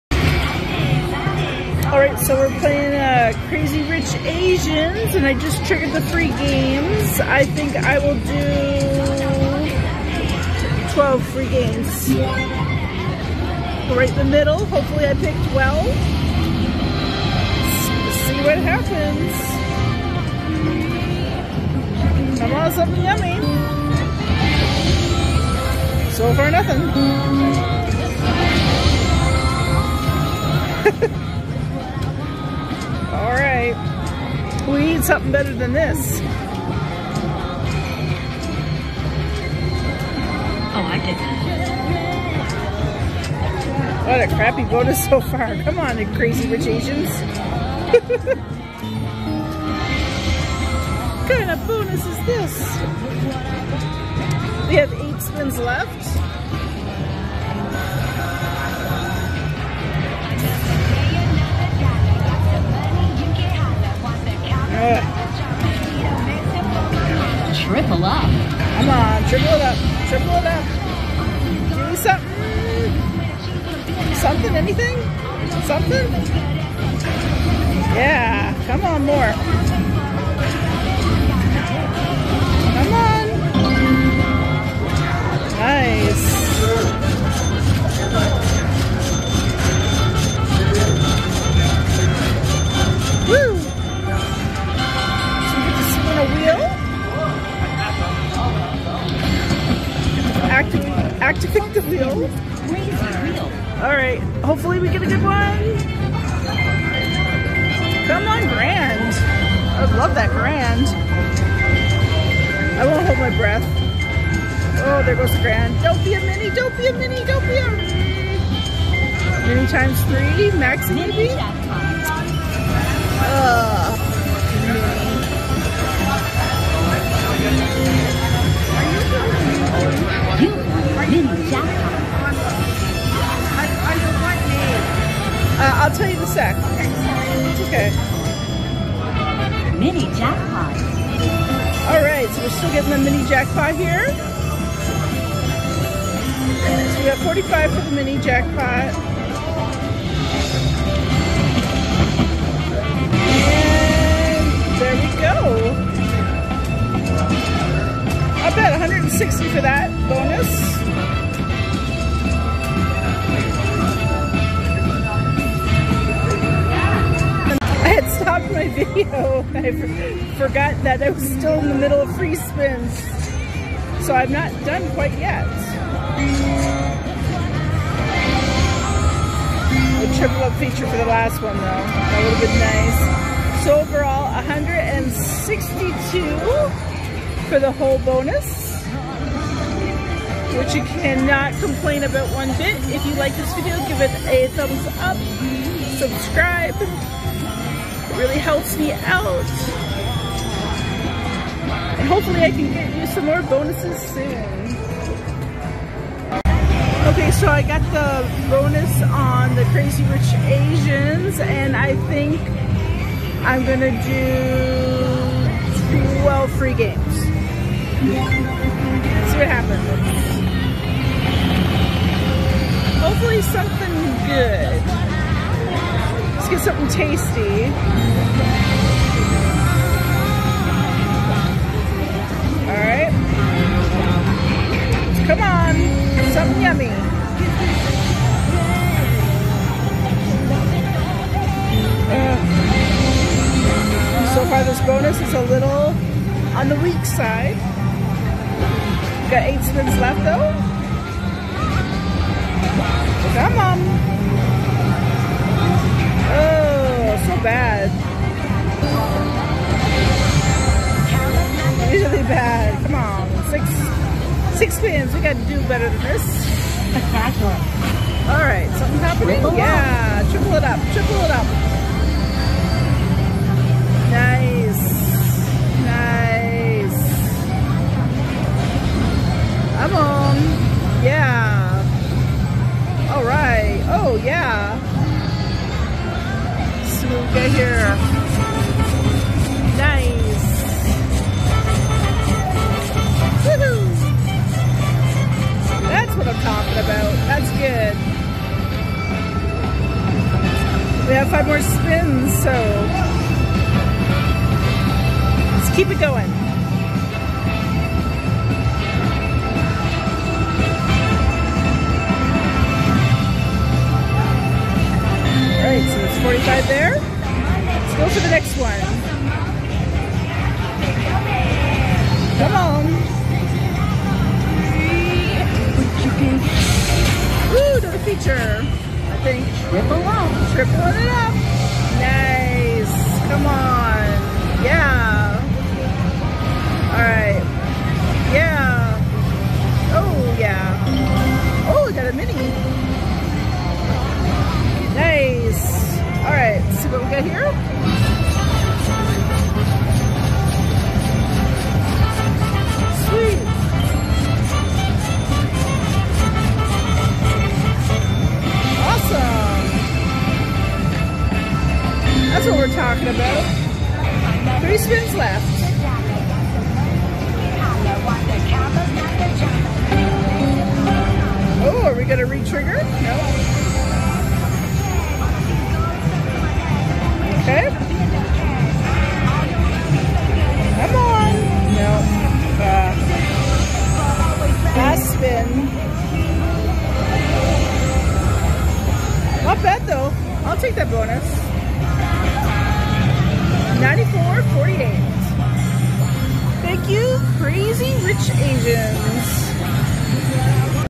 I Was So Excited to Get the Wheel Bonus on Crazy Rich Asians
I was in the middle of filming my Crazy Rich Asian bonus when a lady approached me and asked me if I had a youtube channel. I said yes!  She said which one?  I know all the slot channels.   I said, oh I'm just starting out.  She said never mind then and walked away. haha

Well, I'm still not a big youtuber, but I do hope you'll sit back and watch me have fun on the slots.    If you happen to be that lady that I met that day, Hi!! :)